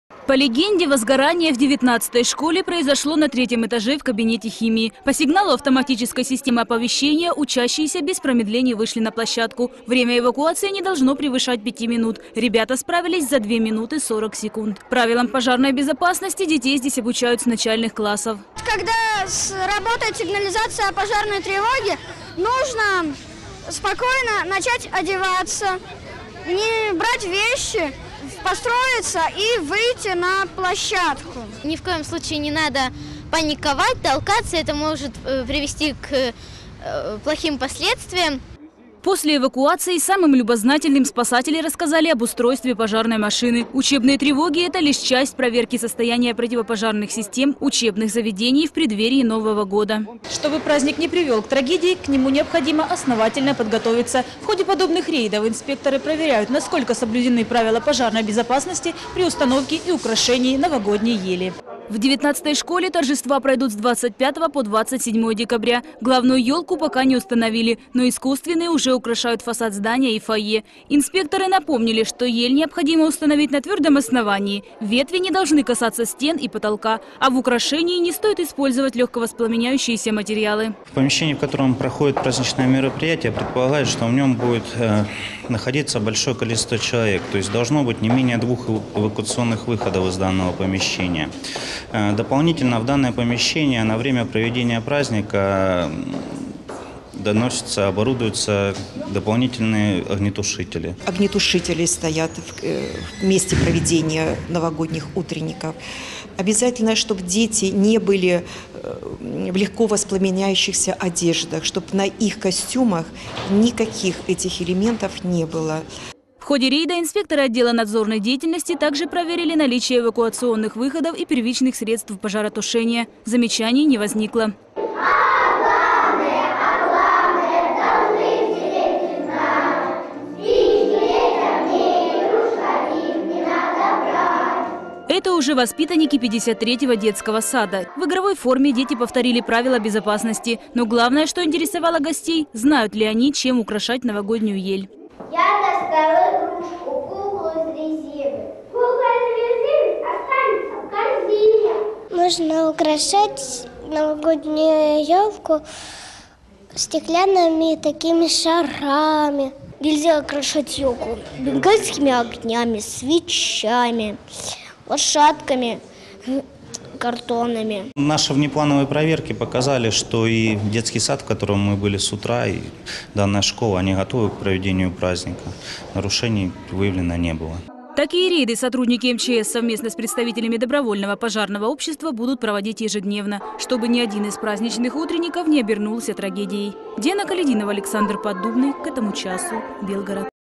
В Белгороде сотрудники МЧС проверяют школы и детсады
Пожарная безопасность городских учебных заведений в преддверии новогодних праздников под контролем. Профилактические мероприятия в городе стартовали в начале месяца и продлятся до 20 декабря.